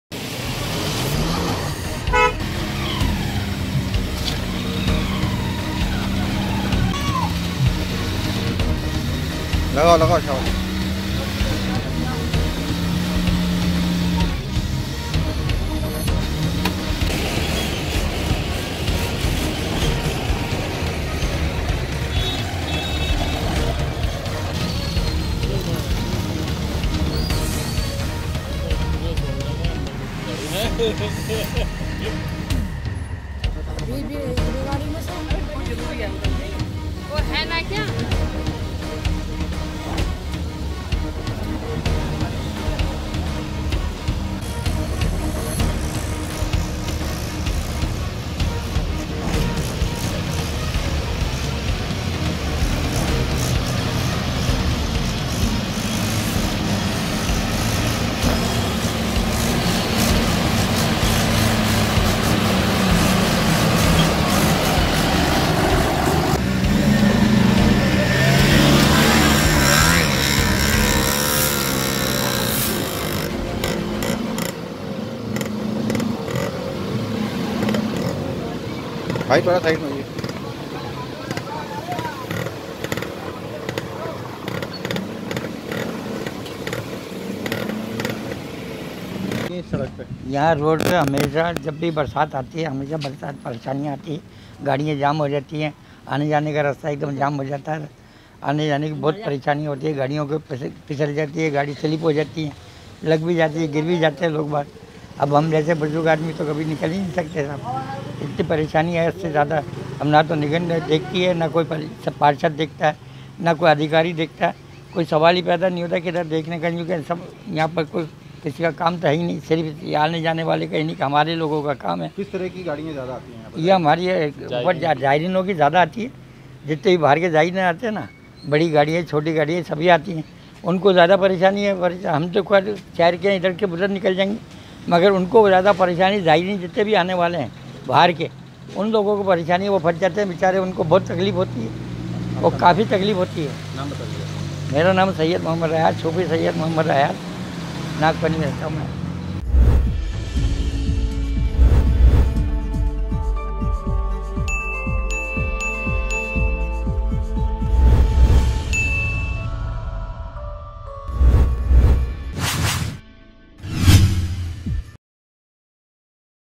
AJMER NEWS | बारिश से प्रशासन के दावों की खुली पोल | MTTV INDIA
बारिश से प्रशासन के दावों की खुली पोल ,बारिश से दरगाह इलाक़े मे फैली गंदगी,पहाड़ियों से गिरने वाले पत्थर सड़कों पर हुए जमा ,क्षेत्रवासियों का प्रशासन पर सफाई व्यवस्था माकूल नही रखने का आरोप